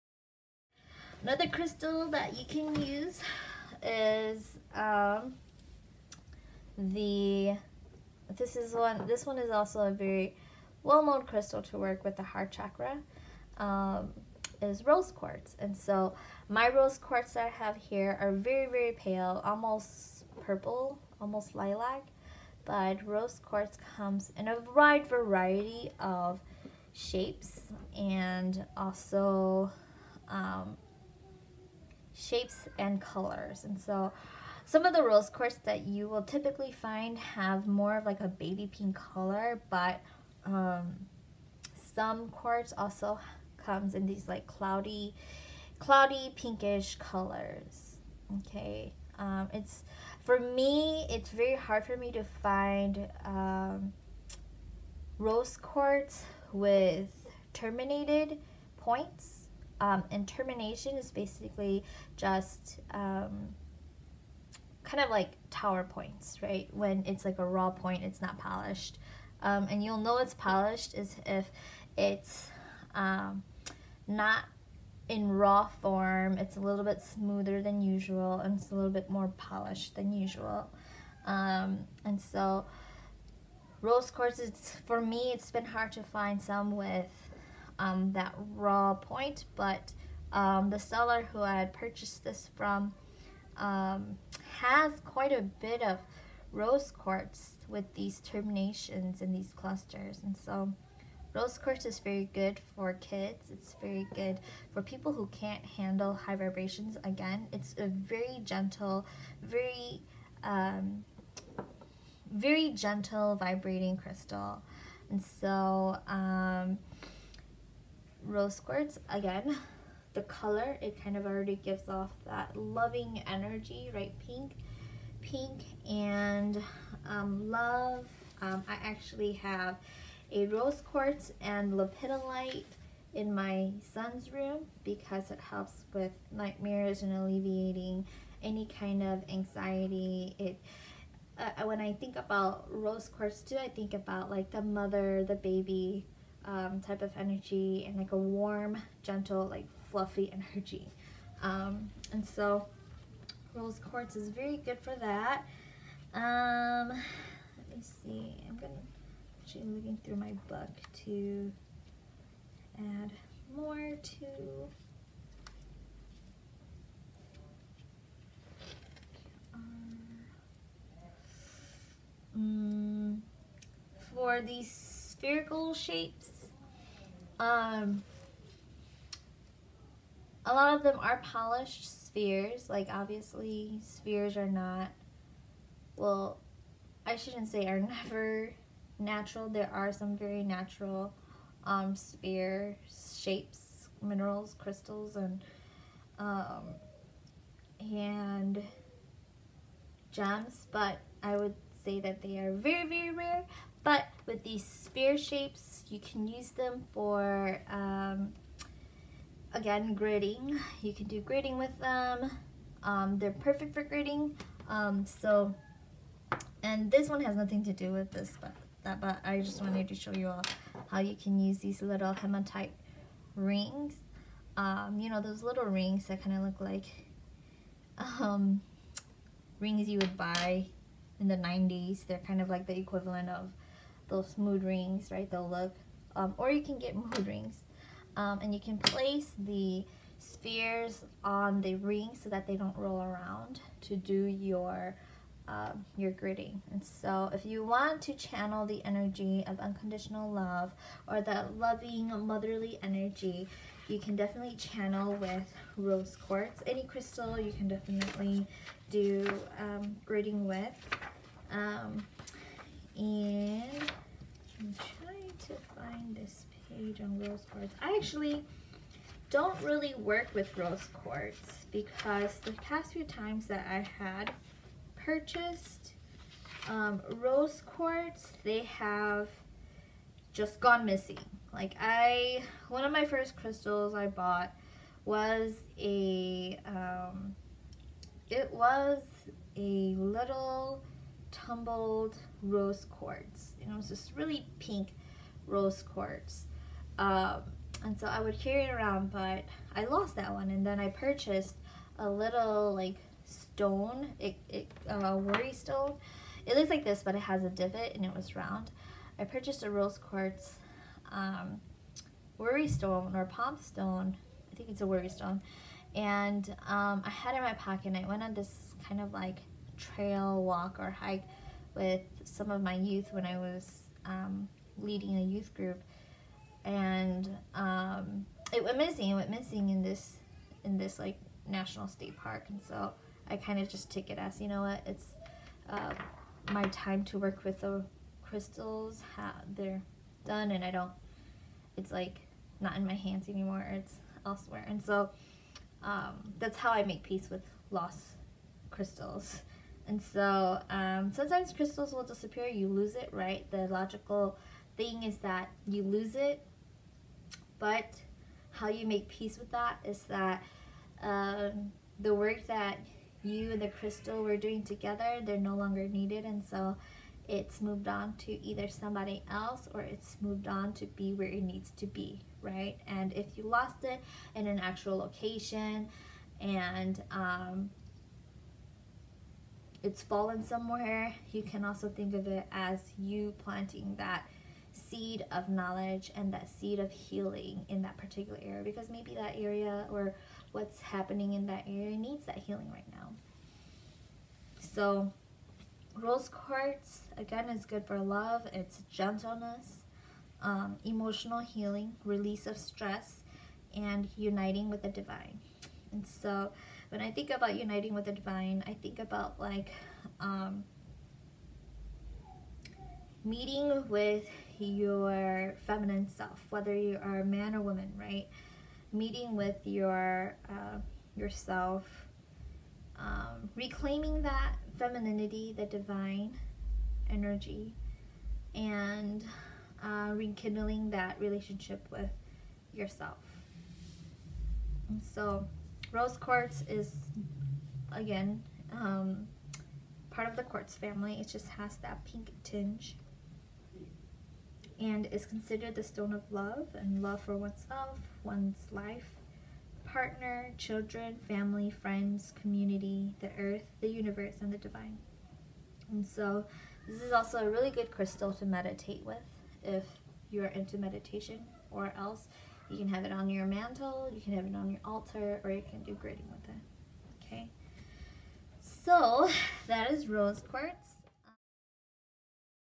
✨⚡️🔮 CRYSTAL SERIES || HEART CHAKRA & SELF HEALING - Rose Quartz🔮⚡️✨
In this video I talk about Rose quartz and what it means when you lose a crystal.

Rose Quartz Healing Properties

“Don’t let the soft pink color and soothing energy of the stone fool you into discounting is power. Rose Quartz is one of the most important stones of our time. It stimulates and opens the heart chakra, clears the emotional body, and assists in the integration and resolution of old emotional programs.

Rose quartz is one of the most powerful stones for the activation of the Human Crystal, precisely because of its heart-healing properties. As we evolve into a new paradigm, our energetic center is moving from the hara point below the naval to the heart chakra. As this shift takes place, it is highly important to clear and strengthen this chakra. The heart center is the strongest generator of light energy in the body—even stronger than the brain. The energy of rose quartz helps the bottom of the heart unfold in to a thousand-petaled lotus of light.”- Ahsian

As always, thank you for the support and views!
Best,
MKV